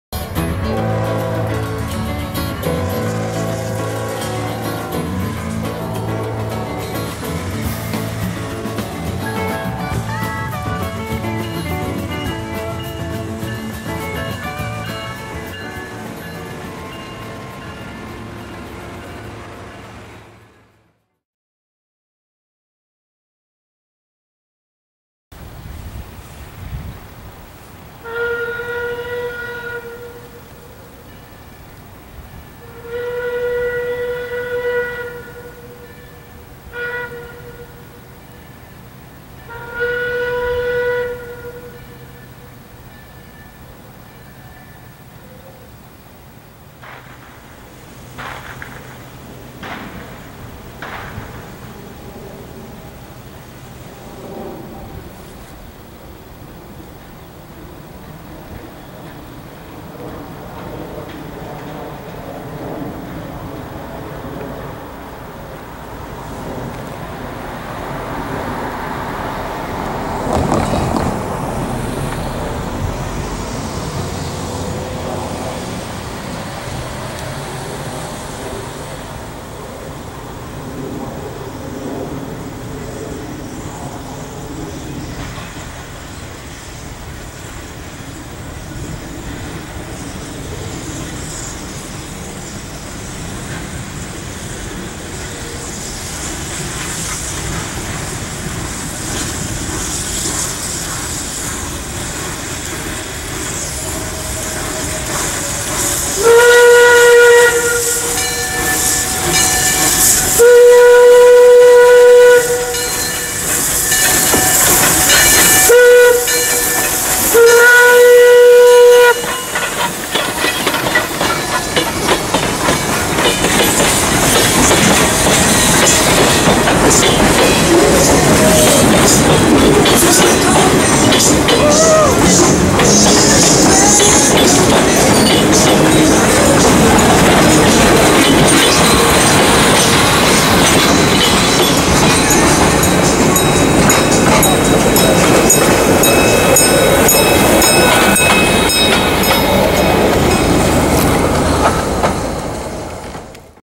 [HD] St. Mary's Steam Train Excursion 2/22/15
Please put in HD for best viewing.
A video from the last day Lehigh Valley Coal Co. 126 ran for the St. Mary's Railroad excursions.